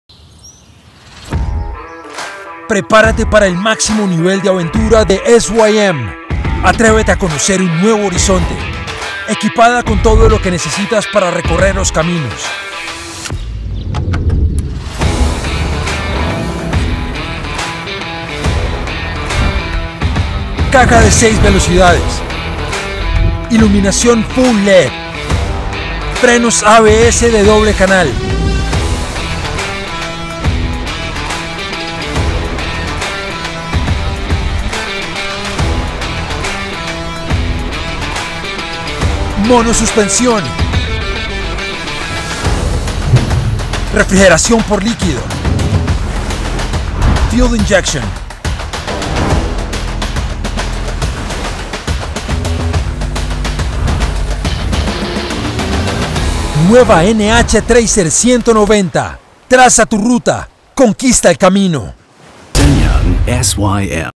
SYM NHT
All new designed for two-wheeled adventure! SYMNH T is equipped with full-LED lighting, liquid-cooled engine, USB charger and LCD instrument.With SYM exclusive innovative technology, the SYMNH T crossover motorbikes make you win at the starting line. To achieve outstanding handling, the SYMNH T features perimeter frame, centered-suspension and front & rear brake disc with ABS.

🎈 The model might differ from the available models. Please see your SYM
      dealers for more details.